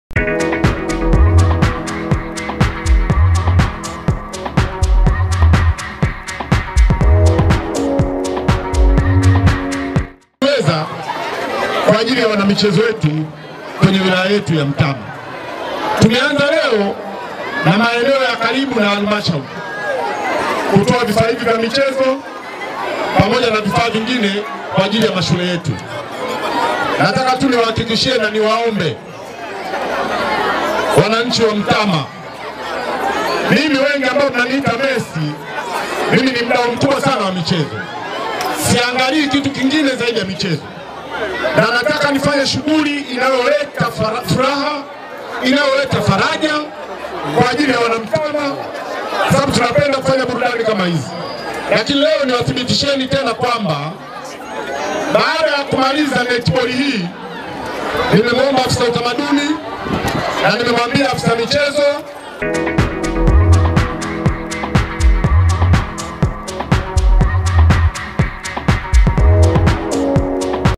WANANCHI WA MTAMA WATAKIWA KUTUMIA MICHEZO KWA AJIRI YA AFYA AJIRA NA FURAHA.
Mdau wa Michezo Selemani Mathew amewataka wanamichezo kuitumia kwa ajiri ya Afya,Ajira na furaha.